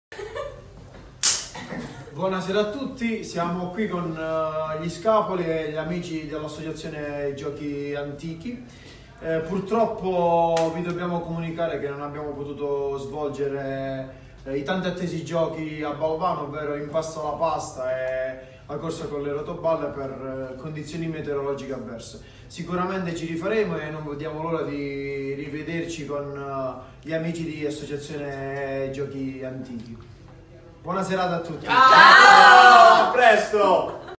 Scapoli Atomic Friends Group per Cibo&Gioco
L'associazione Scapoli Atomic Friends Group saluta AGA a seguito della pioggia che ci ha impedito di assistere alla Corsa con le Rotoballe di Balvano, Potenza, per il progetto Cibo&Gioco - Geoportale della Cultura Alimentare